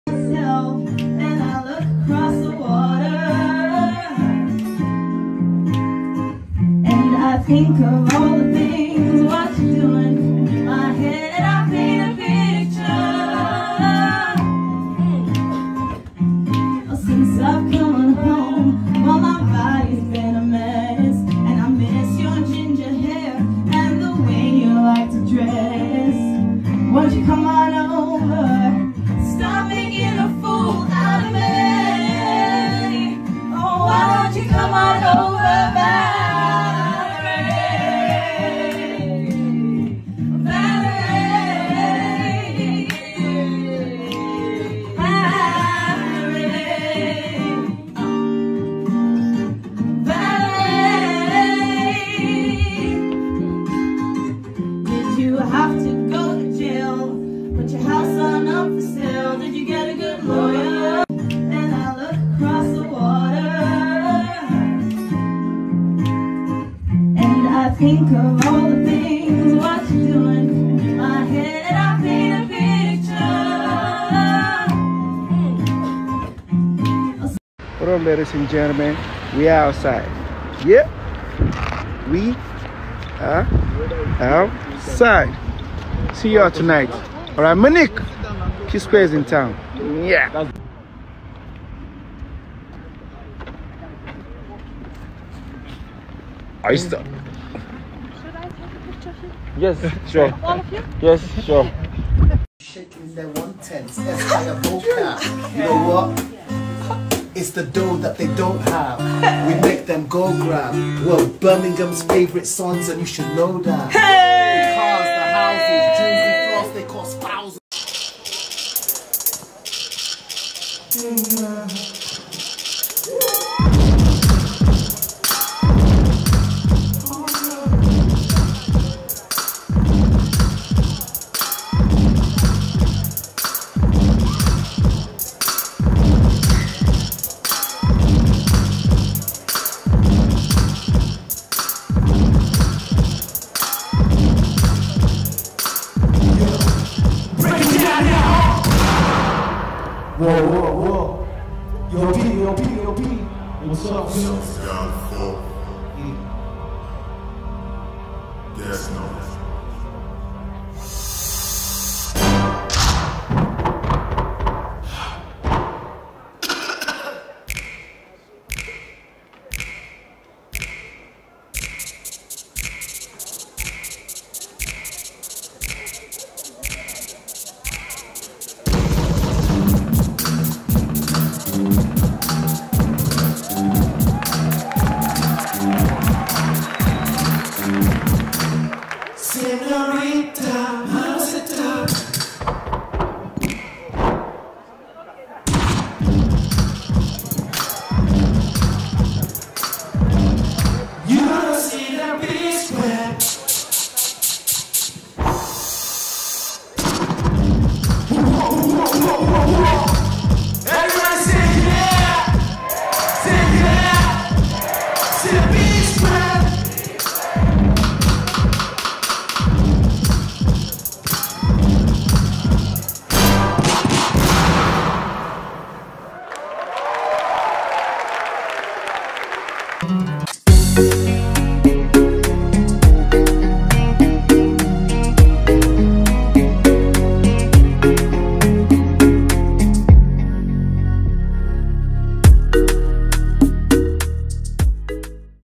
Peter Psquare Mr.P wit joao felix and son camaron in Chelsea vs Everton match,after Germany concert
performance by: @mr.pofficial7601
created/edited by @The_Hadzabe_tribe